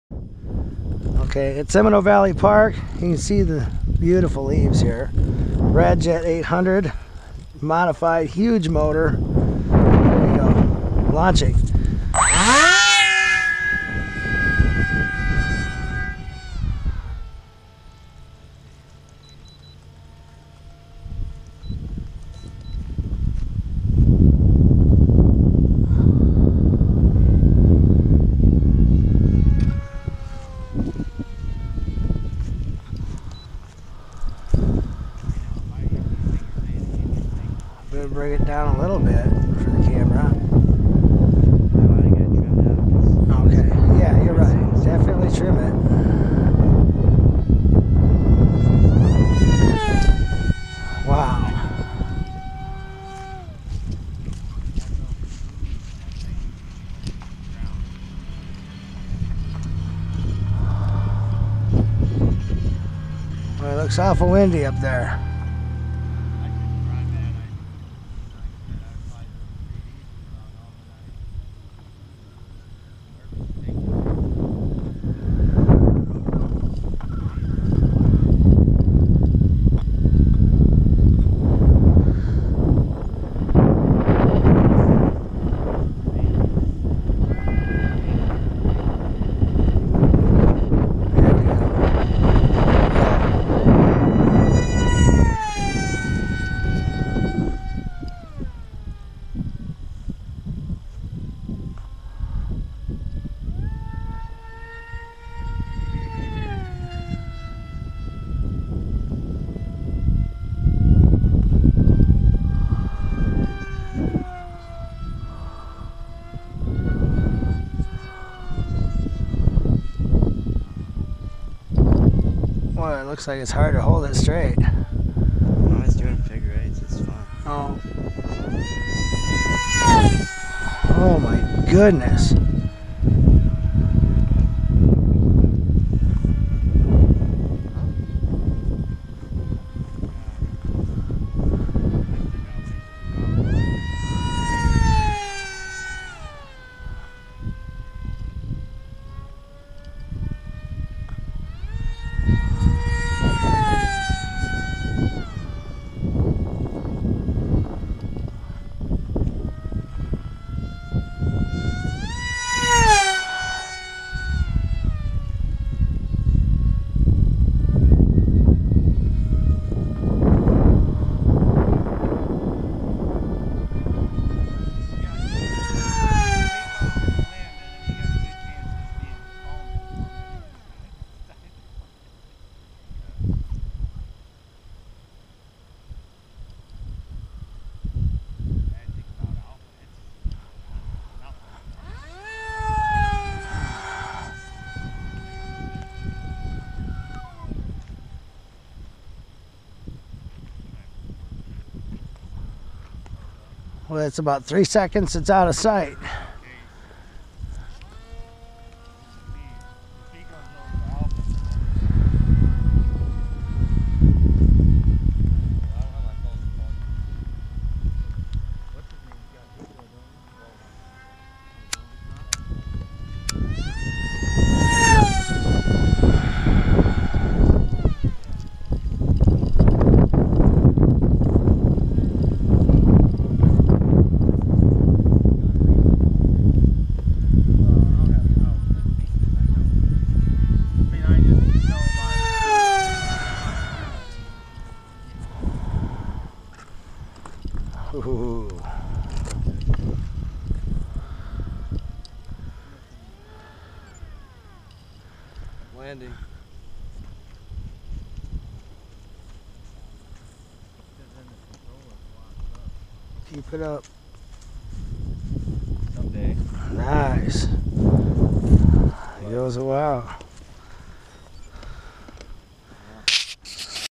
hobbyking radjet modified to go 180mph!
Modified hobbyking radjet 180mph  my son Andrew is the pilot! The boy has skills!